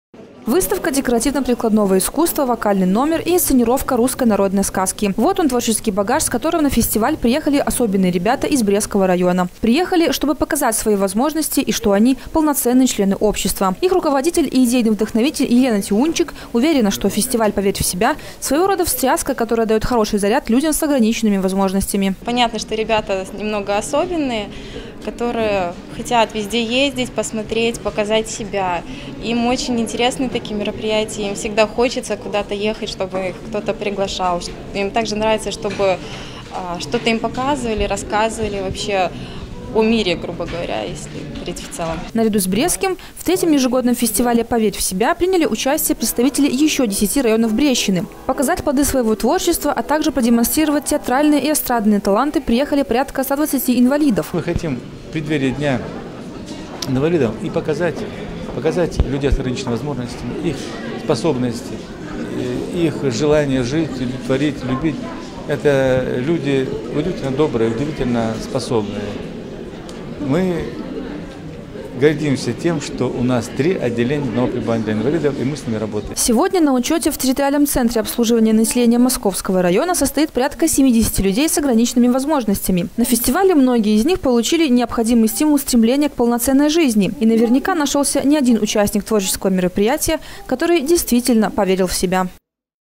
2014-12-03 г. Брест. «Поверь в себя!». Телекомпания Буг-ТВ.
«Поверь в себя!» -  мероприятие, приуроченное ко Дню инвалидов в  Доме ветеранов.
В Республике Беларусь социальная защита людей с различной степенью инвалидности – одно из приоритетных направлений государственной политики. Наверняка поэтому наша страна поддержала инициативу проведенияМеждународного  Дня инвалидов, который мировая общественность отмечает  3 декабря. Сегодня в Беларуси проживает 510 тысяч людей с ограниченными возможностями, в том числе 29 тысяч детей.
В этот день не будет лишним напомнить о том, что среди нас живут люди, заслуживающие не только сочувствия, но прежде всего поддержки, обеспечения их прав на достойную жизнь, труд и отдых. В канун Международного дня инвалидов в Бресте по инициативе Территориального центра социального обслуживания населения Московского района, а также Брестской областной организации «БЕЛТИЗ»прошёл третий областной ежегодный фестиваль инвалидов «Поверь в себя!». Об участниках конкурса и атмосфере, царившей на празднике  - узнаем из материала